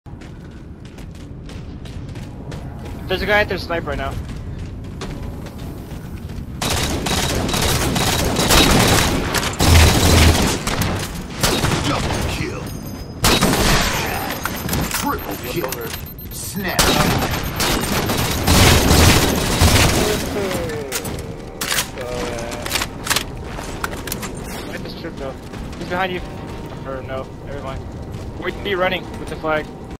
MLG Triple (Over Choke Livecap)
Livecappin' some MLG Customs and got this pretty little ditty on Big Balles and Boomr.